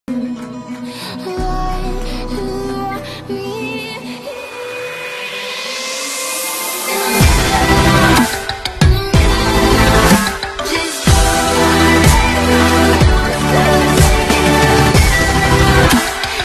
FOTO CATALOG SANDAL dengan SONY A6400 & GODOX TT600
Sony a6400 memang sangat bisa di andalkan naik untuk Foto apalagi Video.